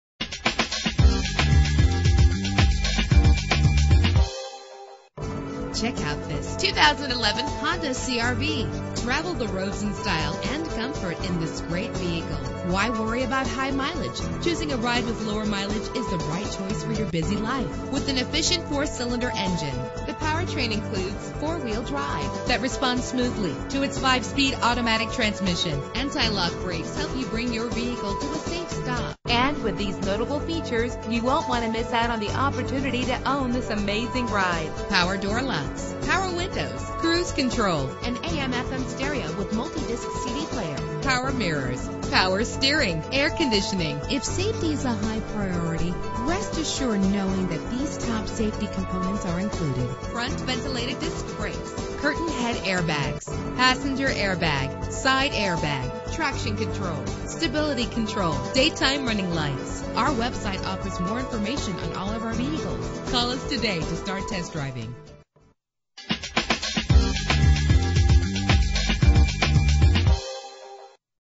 2011 Honda CR-V - WATERFORD PA
Year: 2011
Make: Honda
Model: CR-V
Trim: SE
Engine: 2.4 liter inline 4 cylinder 16 valve
Transmission: 5-Speed Automatic
Color: Gray
Mileage: 10590
Address:
1010 ROUTE 19 NORTH